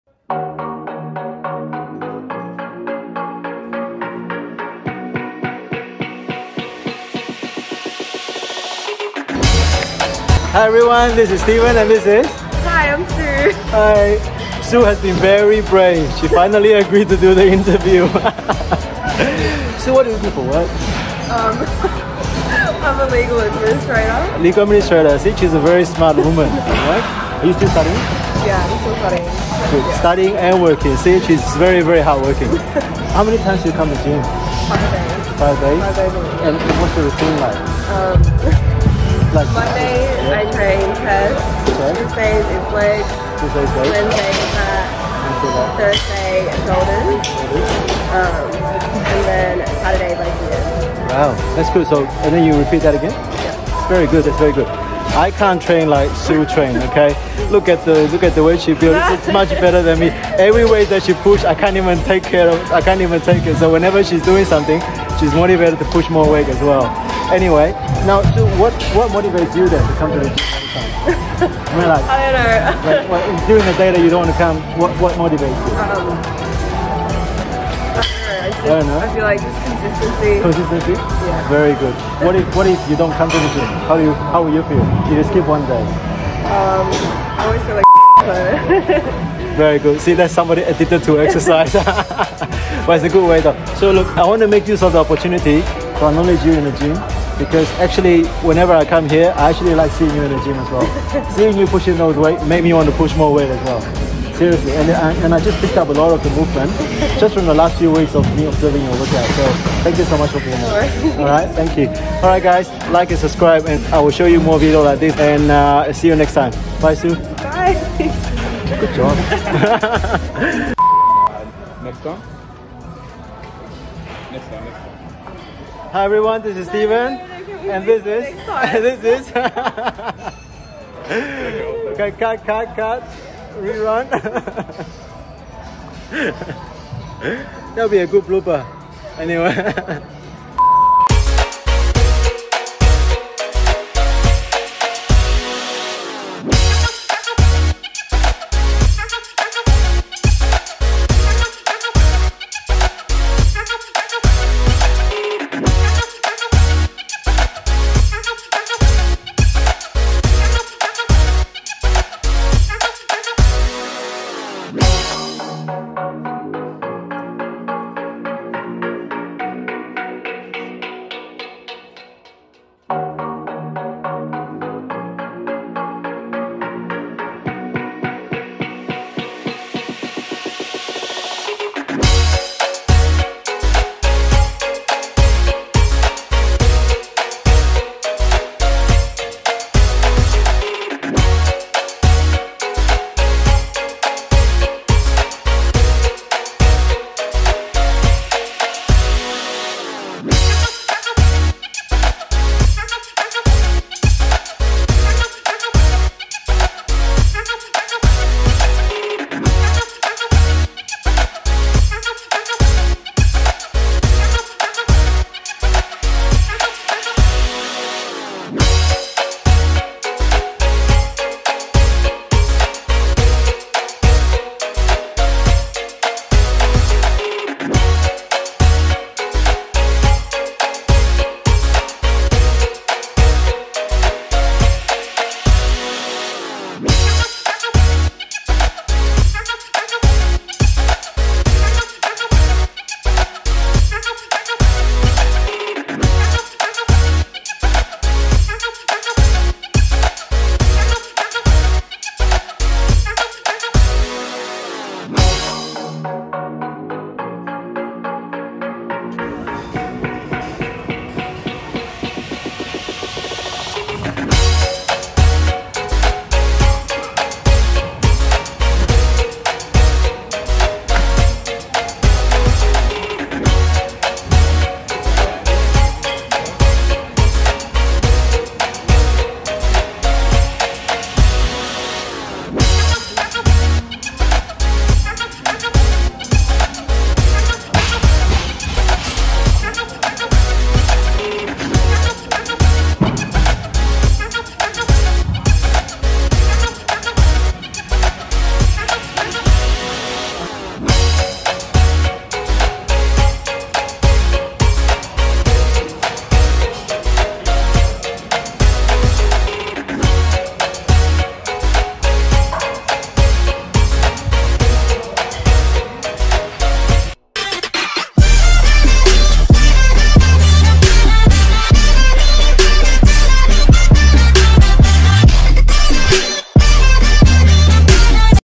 Gym Motivation #10 👊🏼50 Gym Mates in 50 Weeks Series 👊🏼🤜🏼🤛🏼
Make friends in the gym and have fun working out together 💪🏼✊🏼🤜🏼🤛🏼👍🏼

Purpose of this 🎬 video series 🎞 is
* To acknowledge the friends at the gym 🏋🏻‍♀️🤜🏼🤛🏼
* To motivate others ✊🏼 especially those starting (or restarting after long COVID break)👟👟
* To keep the health lifelong journey fun 🤩💪🏼👍🏼🤜🏼🤛🏼💥

50 Gym Mates in 50 Weeks series

Time Stamp
00:00 Gym mate #10
00:19 What do you do for work?
00:32 How many times a week you work out?
00:36 What is your weekly routine like?
01:08 What motivates you to work out?
01:33 Acknowledgement
02:00 Blooper
02:20 Check out her workout